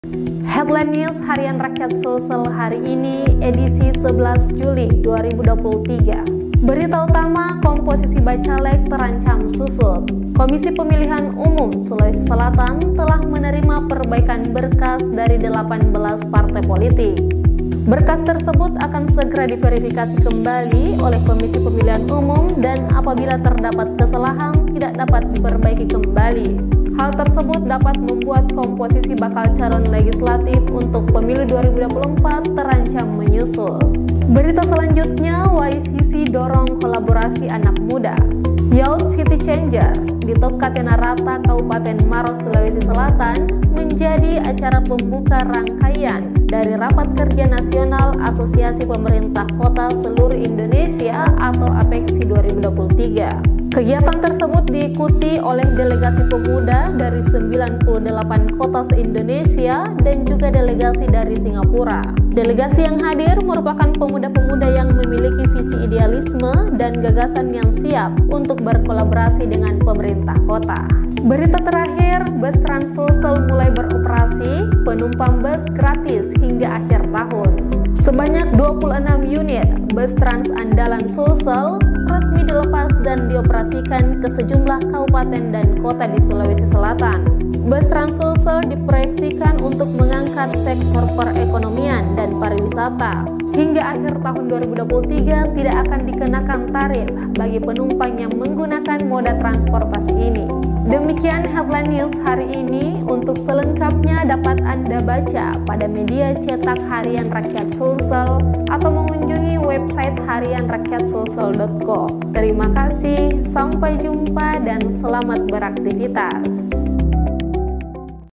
Komposisi Bacaleg Terancam Susut | Headline News
Headline News Rakyat Sulsel Selasa 11 Juli 2023
1. Komposisi Bacaleg Terancam Susut
2. YCC Dorong Kolaborasi Anak Muda
3. Penumpang Bus Gratis Hingga Akhir Tahun